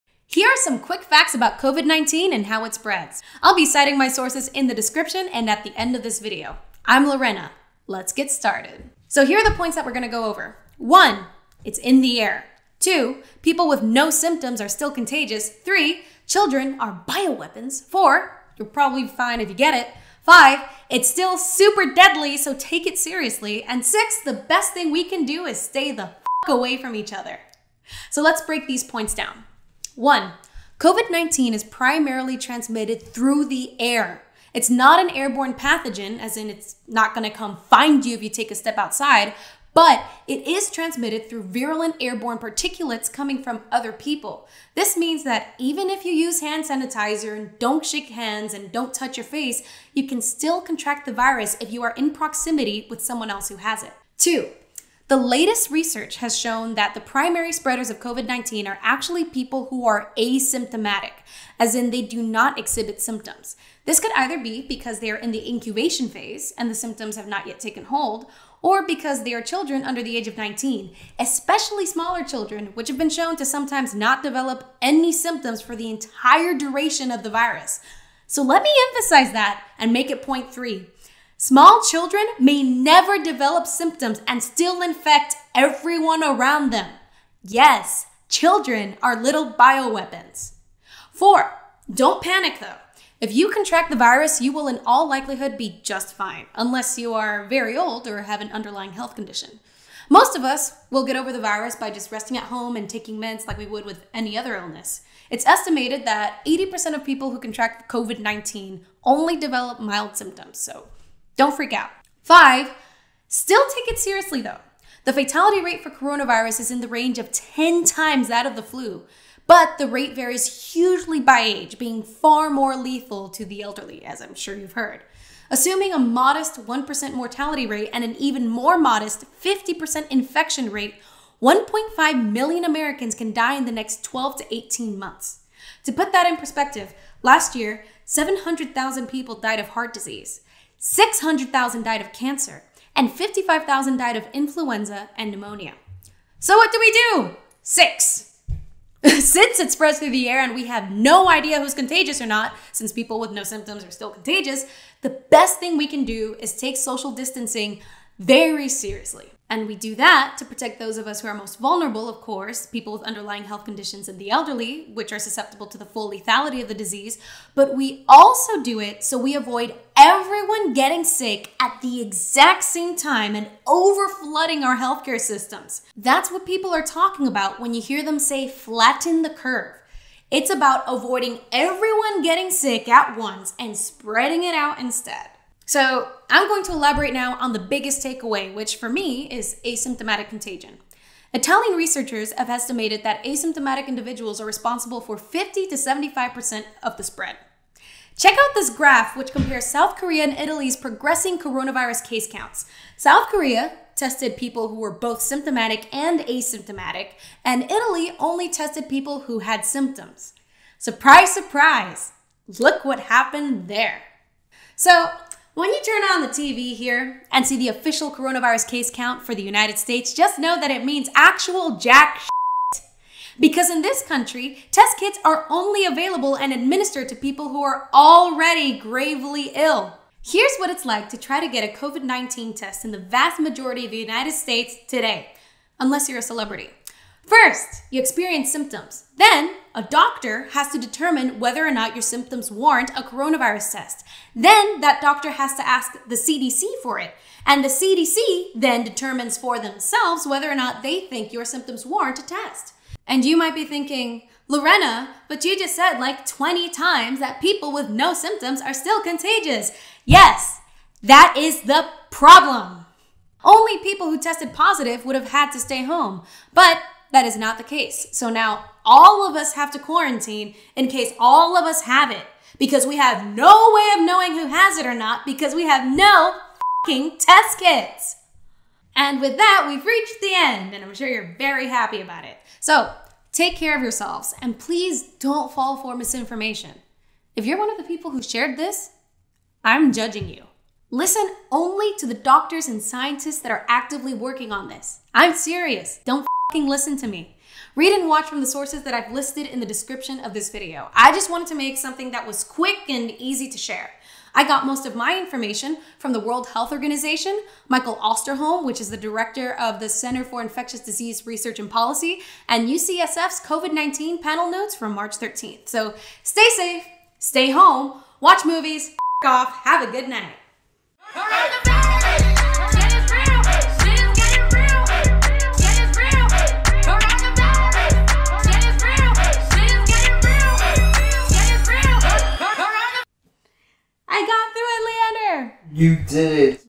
COVID-19 Quick Facts | Clearly Stated
The most important facts about COVID-19 made quick and easy to understand!